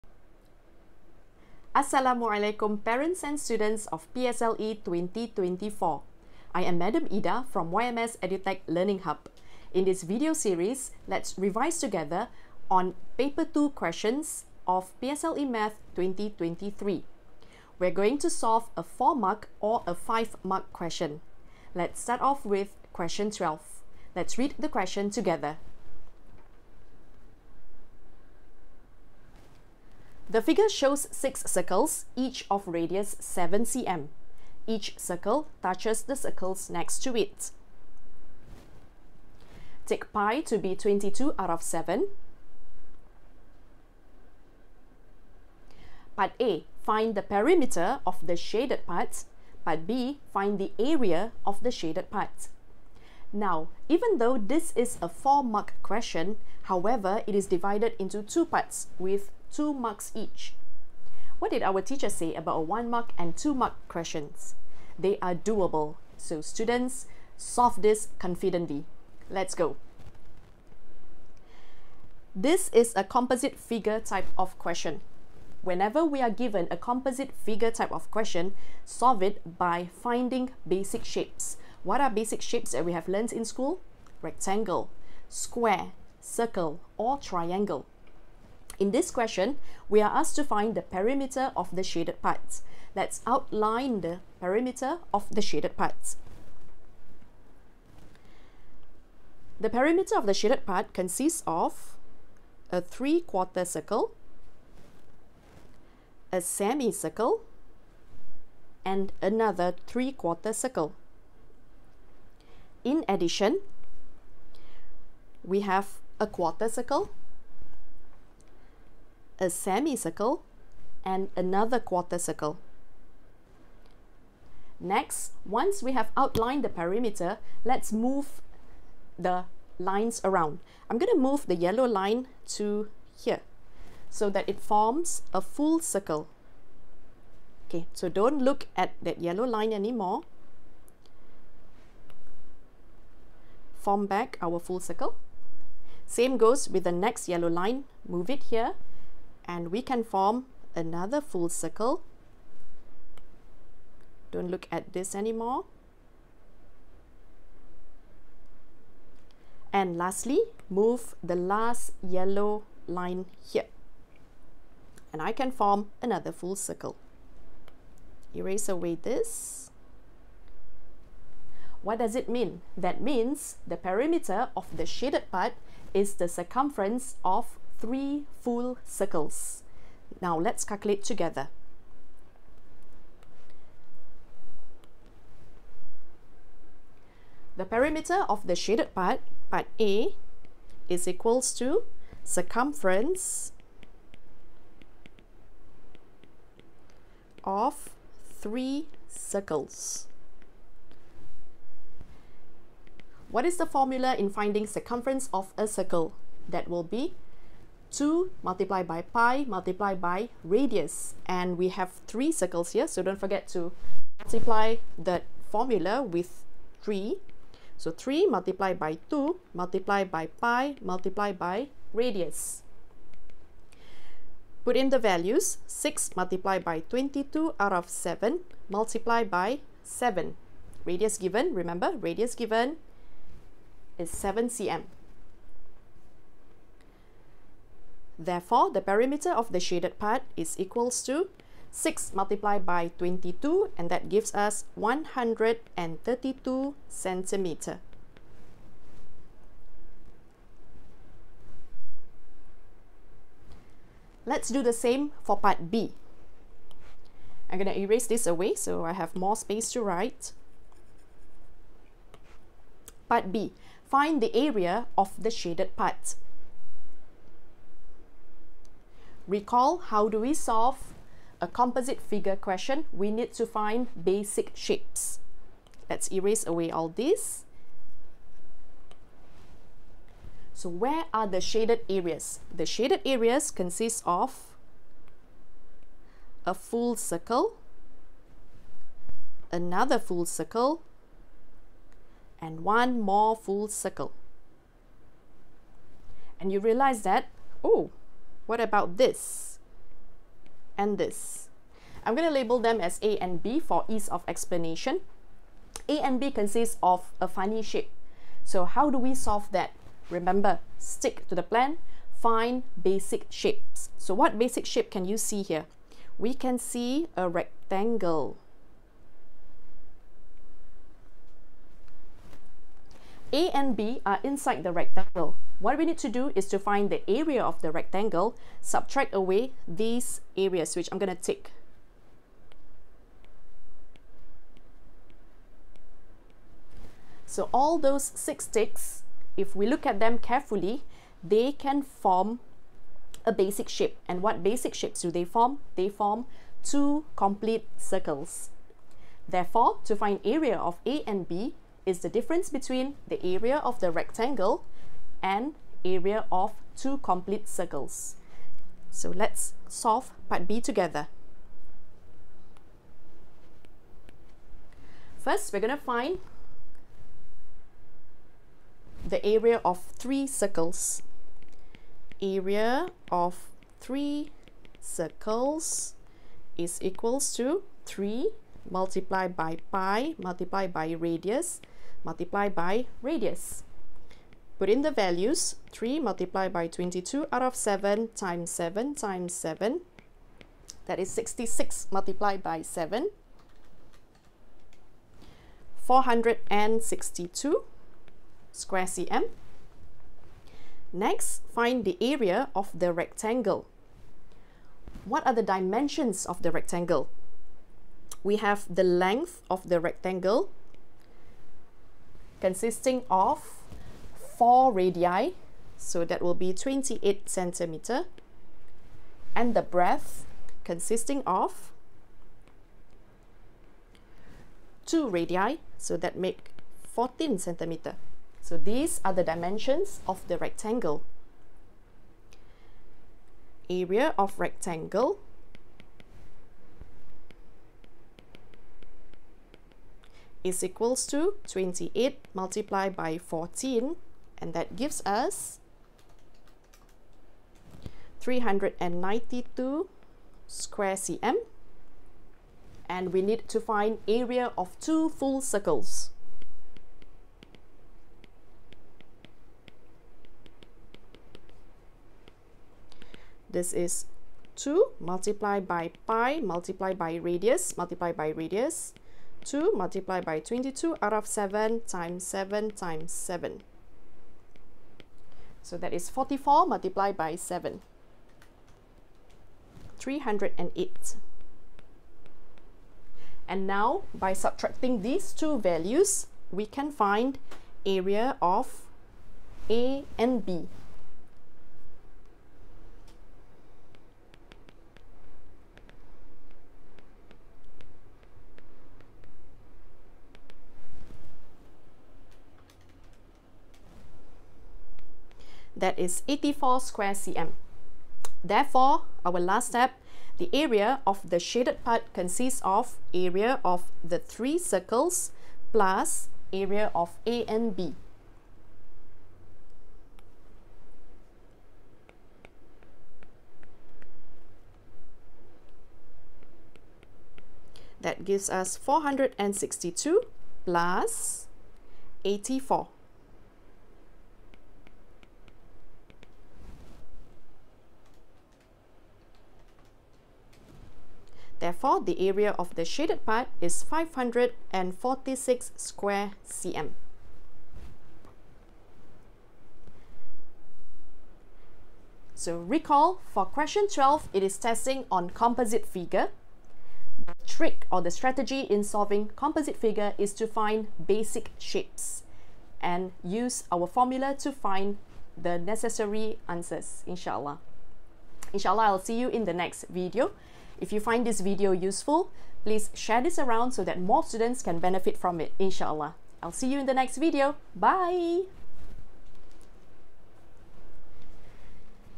PSLE MATH 2023 PAPER 2 Q12
Solve PSLE Math composite figure question easily. Watch to find out the secret to the ease of solving.